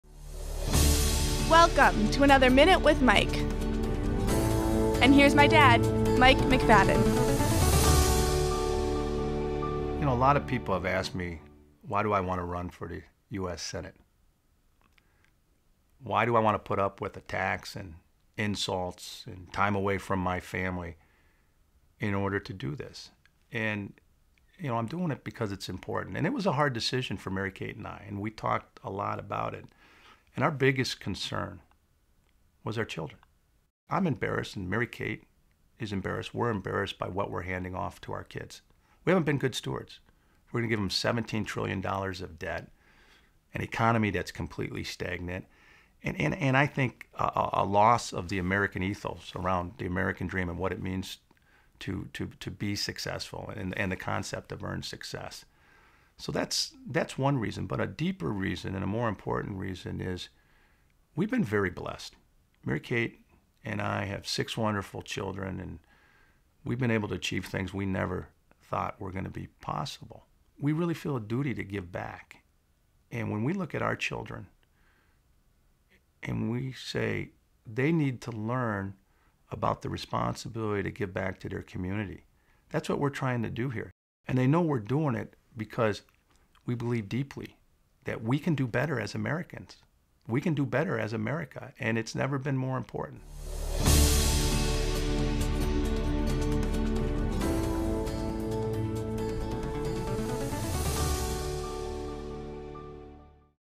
Minute with Mike: Sense of Duty
In the first "Minute with Mike" video, U.S. Senate candidate Mike McFadden discusses why he is running against Sen. Al Franken (D-MN).